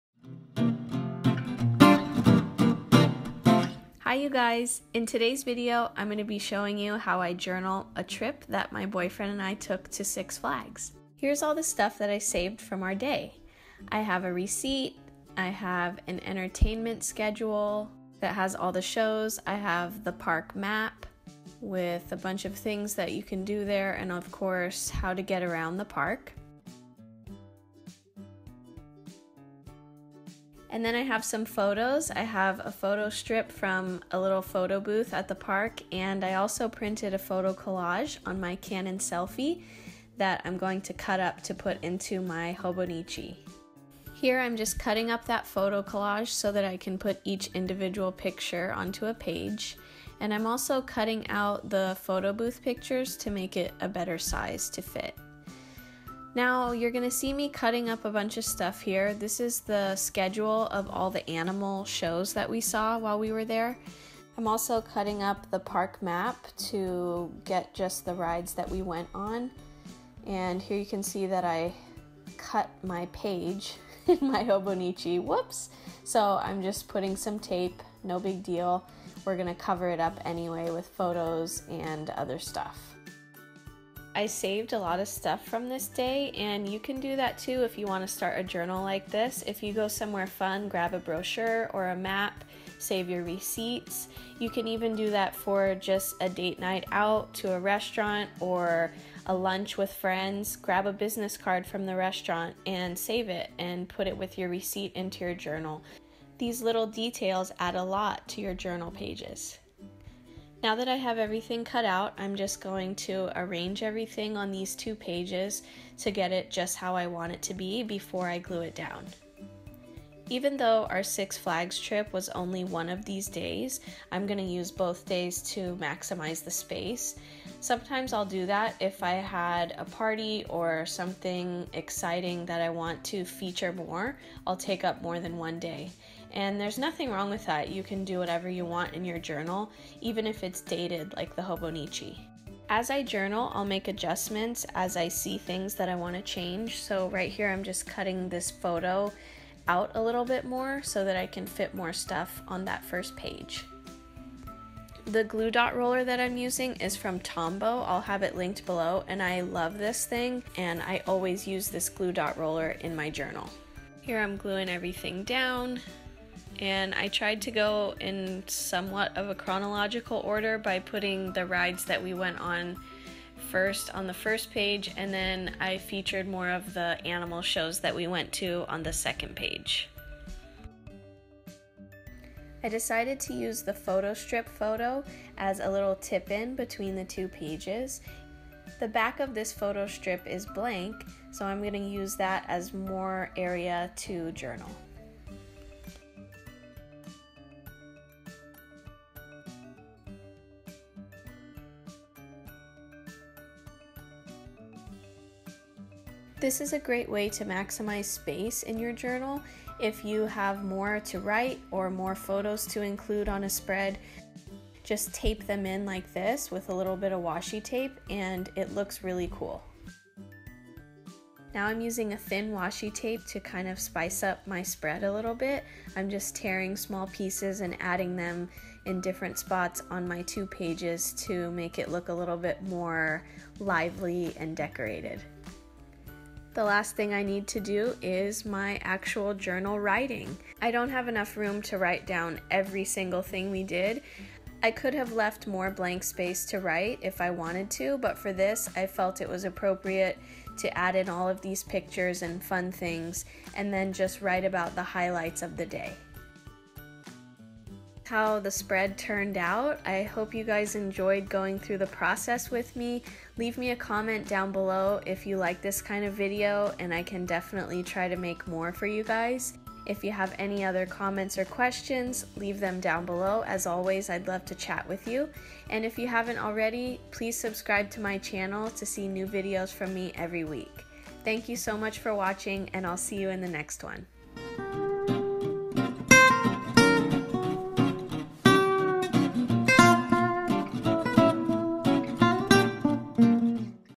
Journal With Me
Check out my journaling process in this journal with me video! I show you what I do to journal a special event, in this case - a day trip to Six Flags Discovery Kingdom! My daily journal is a Hobonichi A6 (Japanese version). I used a variety of supplies, which I will list below for you guys to check out.